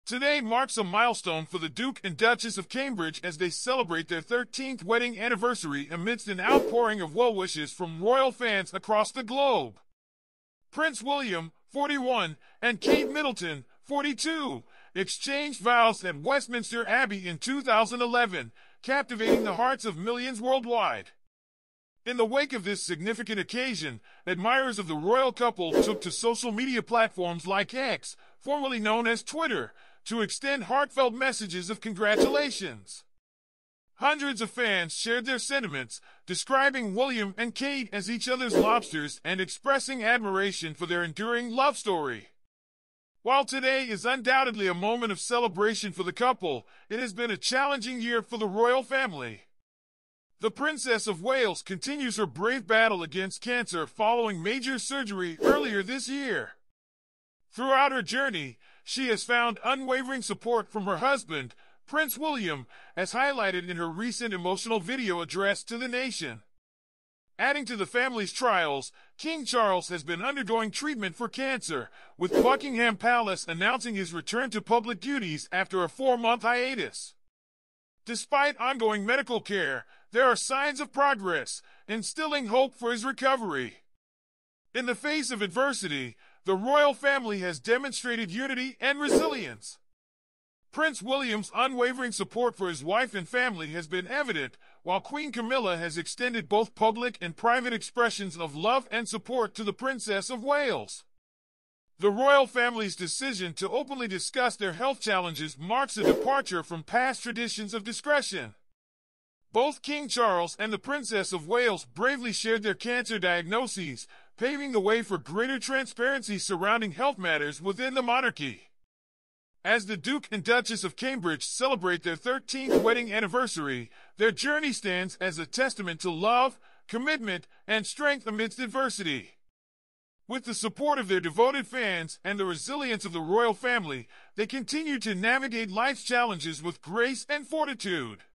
Royal Love and Resilience: William and Kate's 13th Anniversary Celebration!
Today marks a milestone for the Duke and Duchess of Cambridge as they celebrate their 13th wedding anniversary amidst an outpouring of well-wishes from royal fans across the globe. Prince William, 41, and Kate Middleton, 42, exchanged vows at Westminster Abbey in 2011, captivating the hearts of millions worldwide.